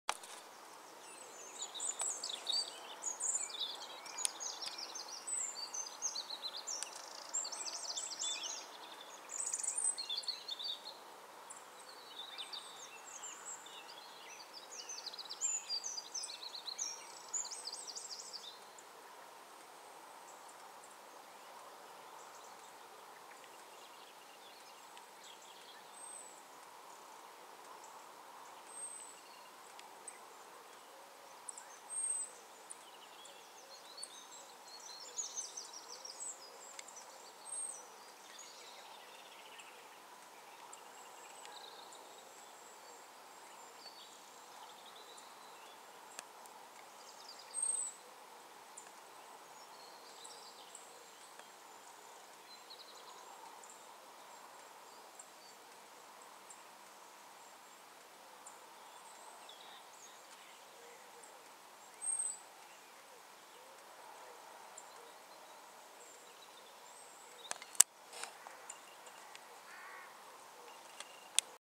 Bretagne,Lever du soleil,( ploudaniel) 02 10 11.
Encore! Encore! qu'elle est belle nôtre Bretagne sous le soleil 19° le matin 28° après-midi en octobre.